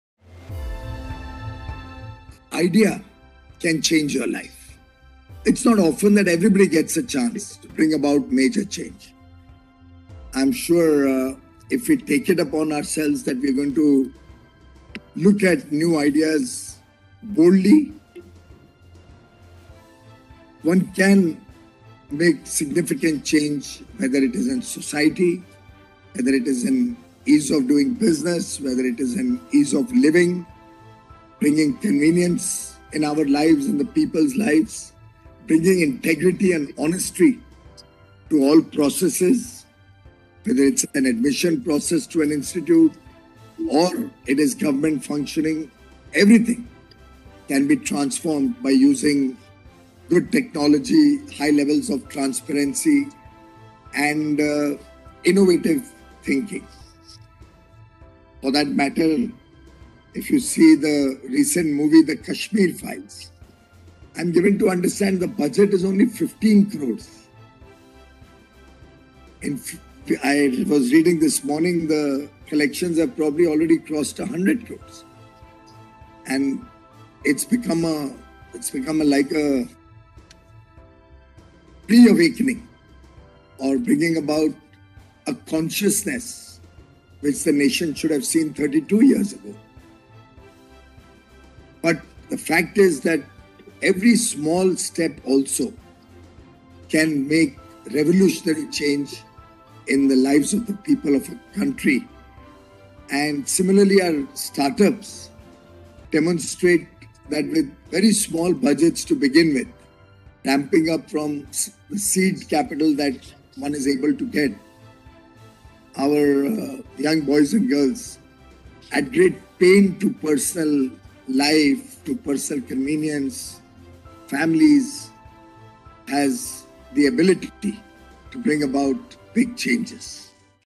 The Kashmir Files Bringing About Consciousness
The movie ‘The Kashmir Files’, with a small budget, has already crossed the 100 crore mark.

It is bringing about a consciousness that the nation should have seen 32 years ago.